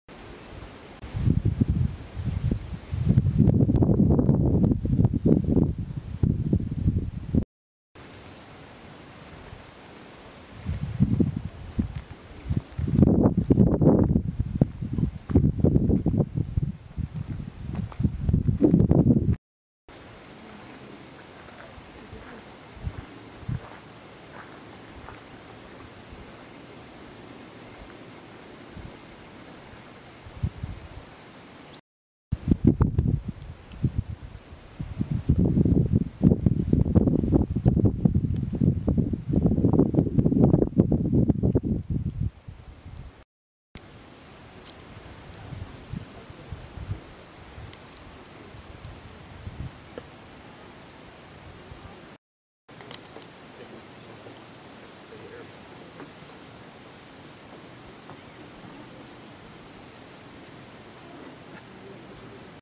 red shouldered hawk perched in a tree (HD)
filmed at brazos bend state park 9-18-11